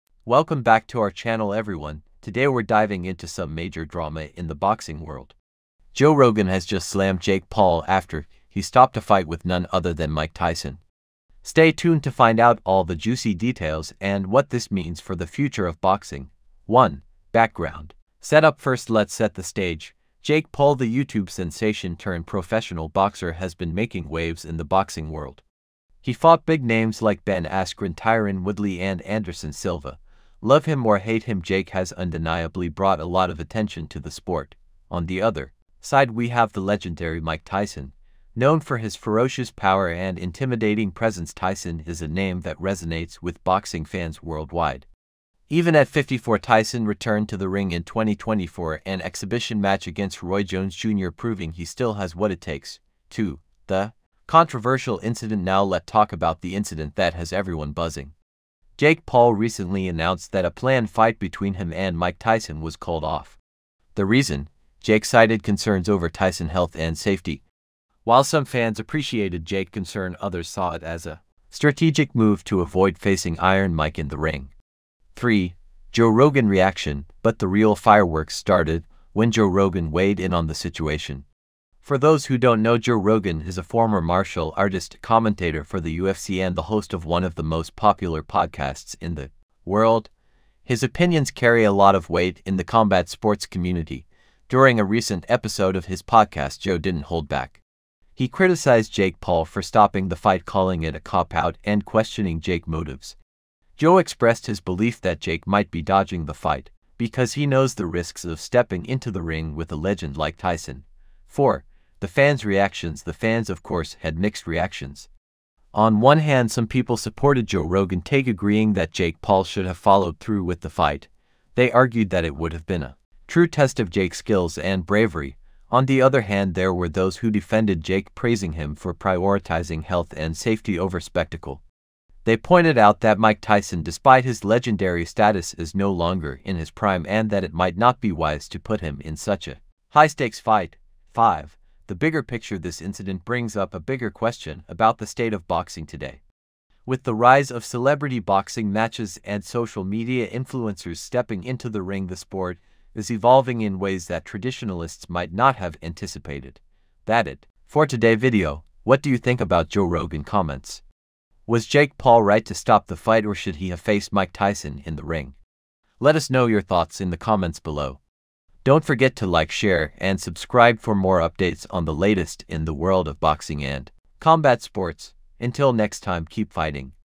Joe Rogan Just SLAMMED Jake Paul After He STOPPED Mike Tyson Fight!
🔥 Drama Alert! 🔥 In today's video, we dive into the explosive comments made by Joe Rogan about Jake Paul after he stopped the highly anticipated fight with the legendary Mike Tyson. Is Jake Paul dodging a fight with Iron Mike, or is he genuinely concerned for Tyson's health and safety? 🤔

Join us as we explore:

The controversial decision by Jake Paul to call off the fight
Joe Rogan's unfiltered reaction and criticism
The fans' divided opinions on the matter
The bigger picture of how celebrity boxing is changing the sport
This incident has sparked a heated debate in the boxing community, and we're here to break it all down for you. Don't miss out on the action and the insights from boxing experts and fans alike!

Timestamps:
Intro
Background Setup
The Controversial Incident
Joe Rogan's Reaction
The Fans' Reactions
The Bigger Picture
Jake Paul's Perspective